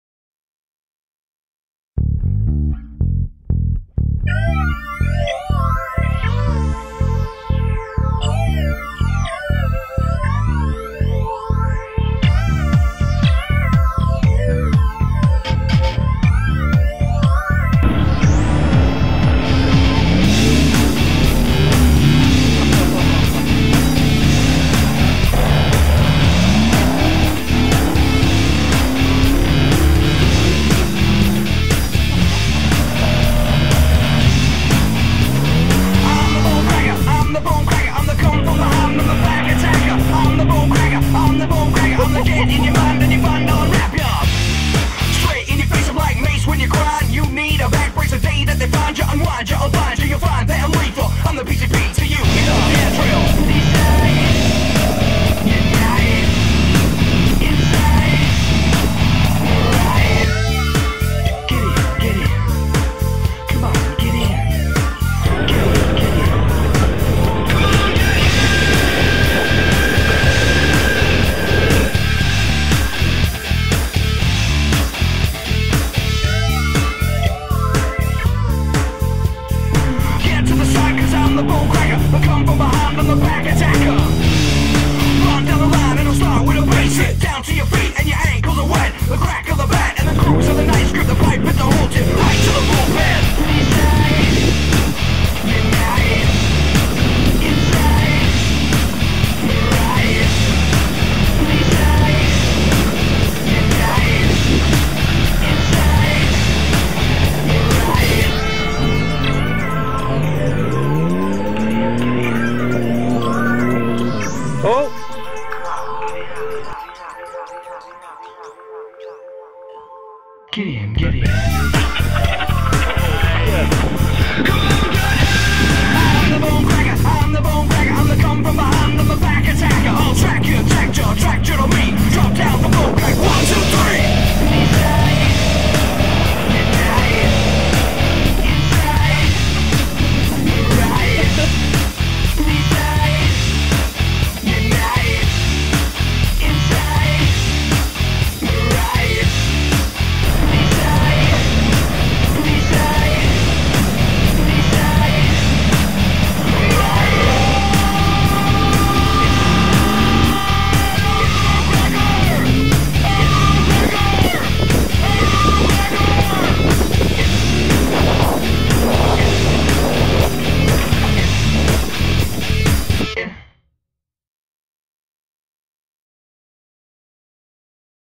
WORE Oct 18 Trip to Anola Pits
Get together in Anola, MB Canada. Ended up having a bunch of full size V8s show up for a change! Lots of power! Lots of wheel spin!